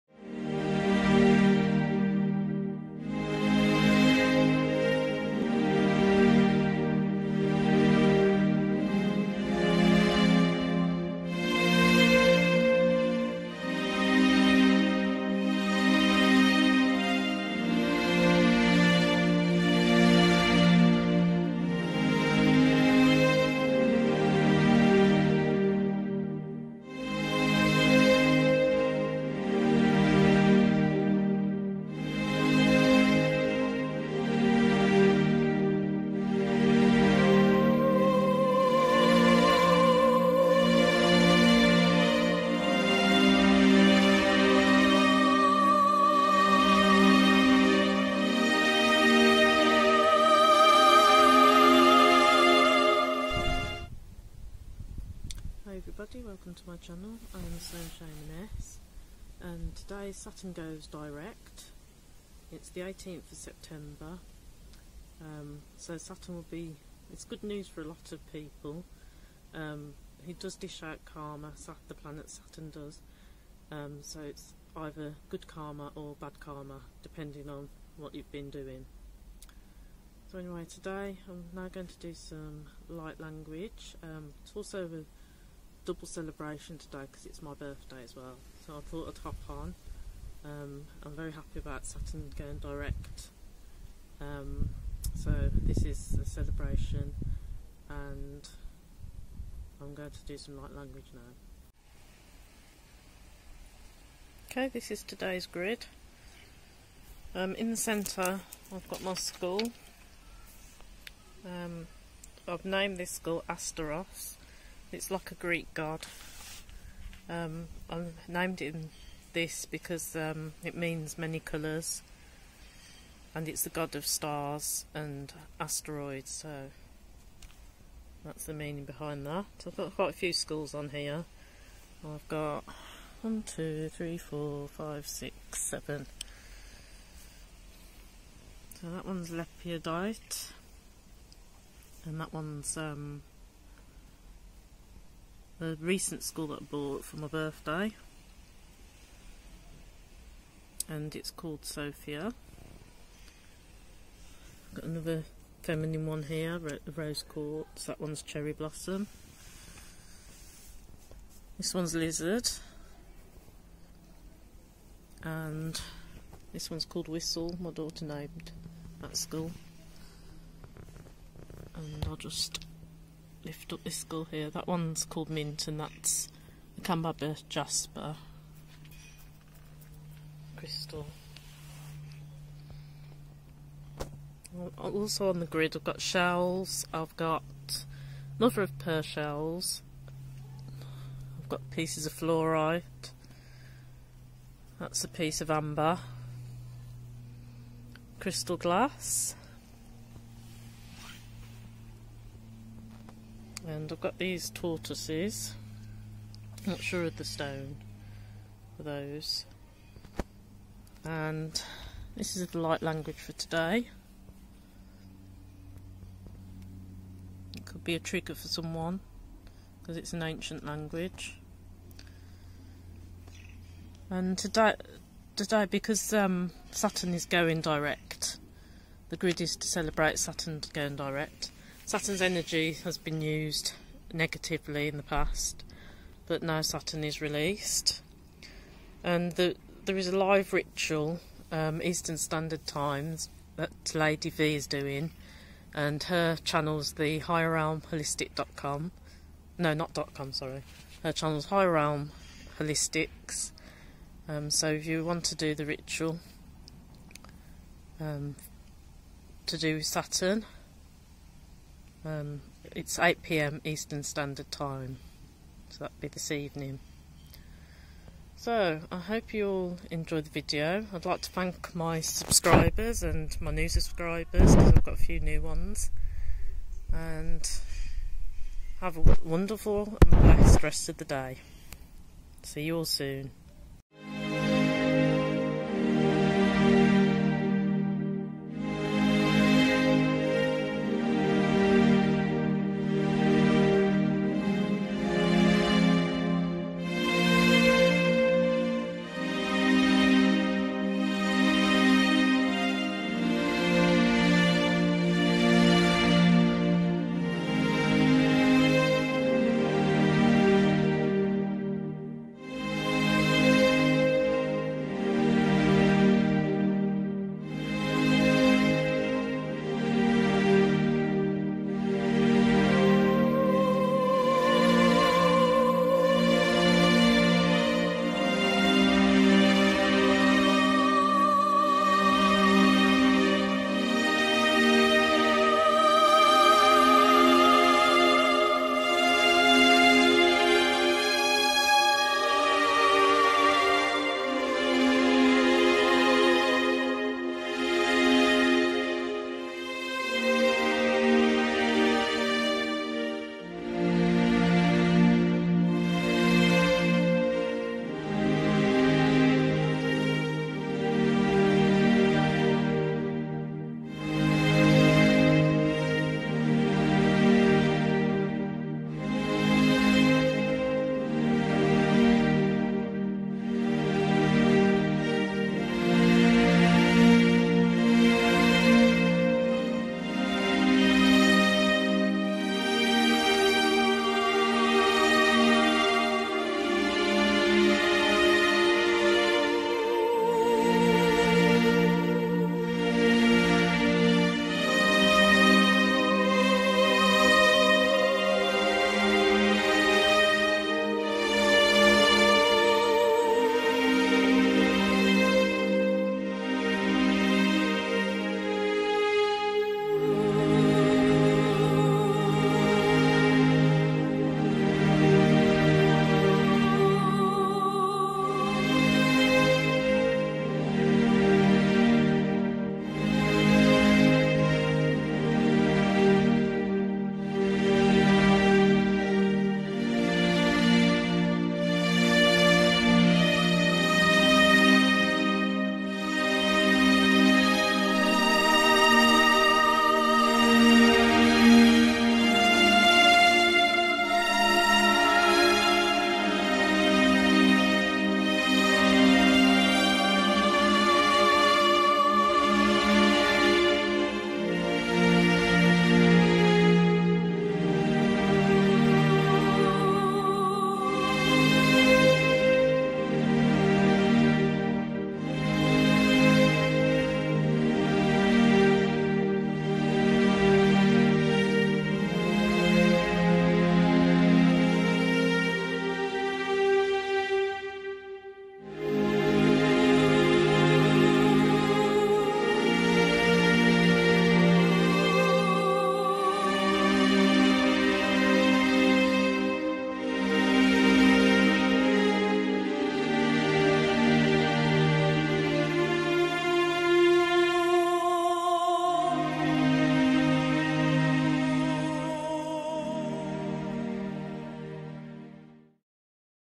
Saturn Goes Direct September 18th, Cosmic Heart & Solar Plexus Light Language Transmission
I create a grid for Saturn and do a light language transmission for the cosmic heart and the solar plexus.